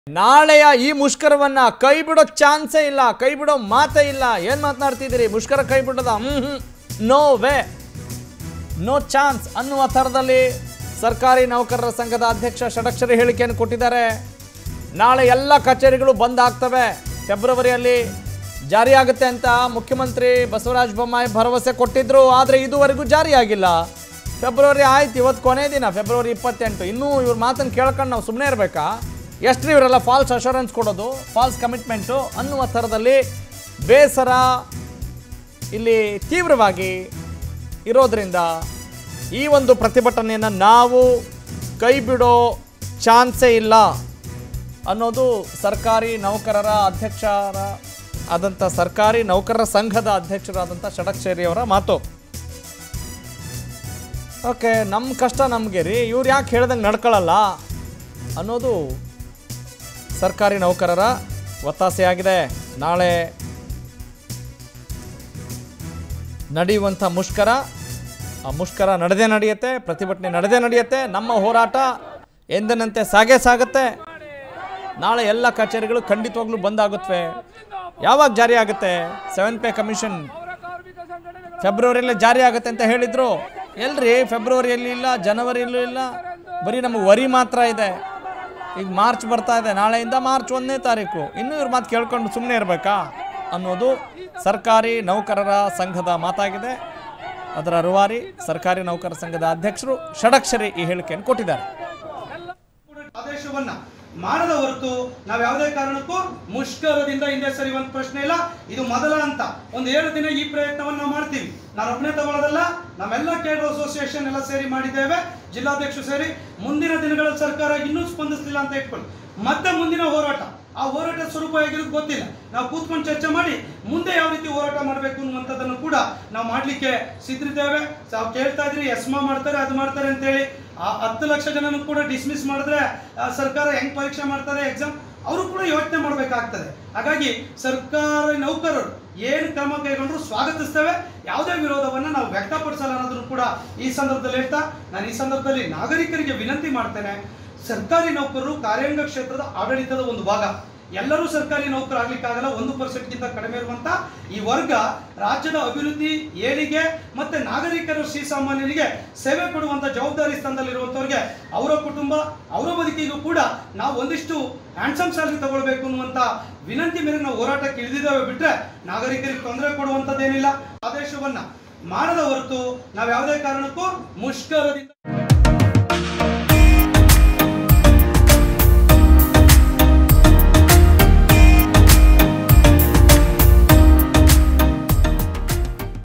Govt Employees Strike : ನಾಳೆಯ ಮುಷ್ಕರ ಕೈ ಬಿಡುವ ಮಾತೇ ಇಲ್ಲ | CM Bommai | Power TV
More Updates Keep Following Us